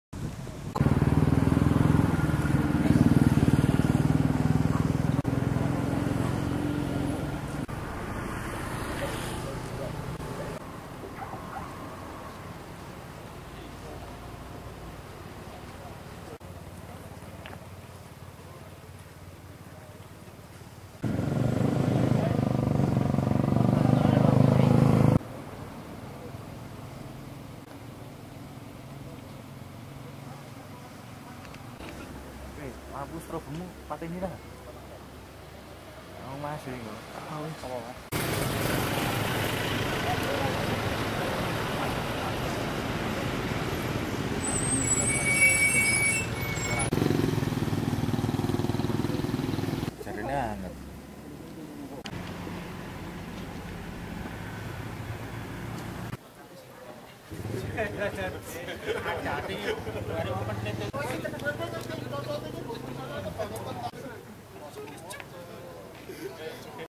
KEBAKARAN GUNUNG LAWU
Titik api masih terlihat di kawasan hutan Gunung Lawu.Api dari kebakaran hutan gunung lawu di Cemoro Sewu menjadi tontonan warga..Sejumlah relawan juga turut siaga..